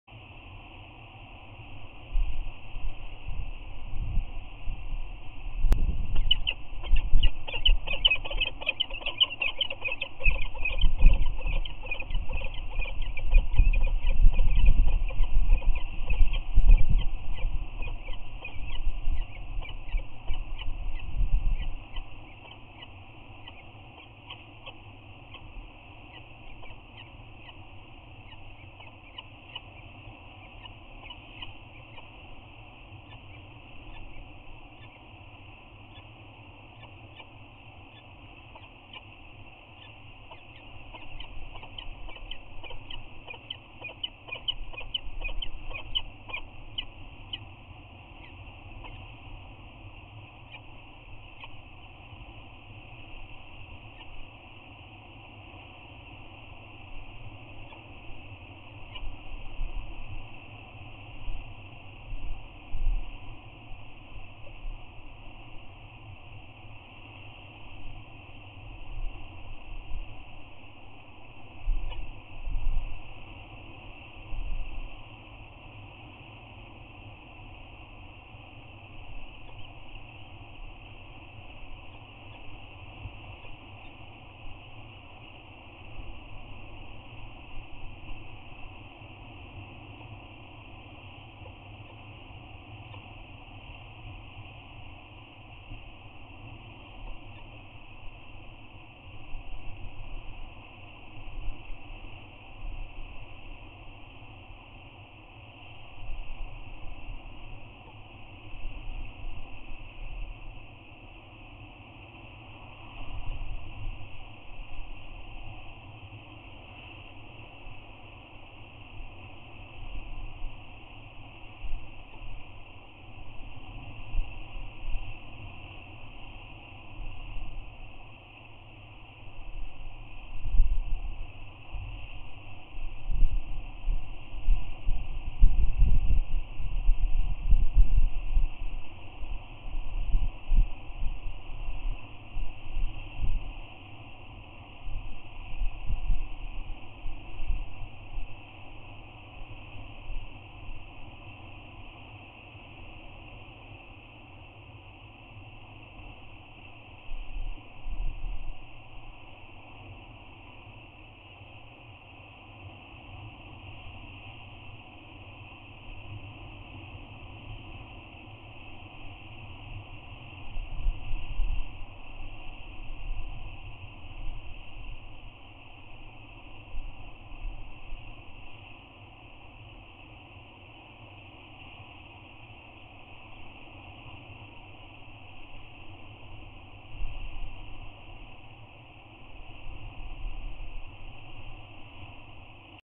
3-8-19 Anacapa Peregrines- Both Visit The Eyrie
Both adults visit the eyrie with their chirping greeting and head bows.  The male flies off and the female back kicks the scrape.  It's much deeper than last year!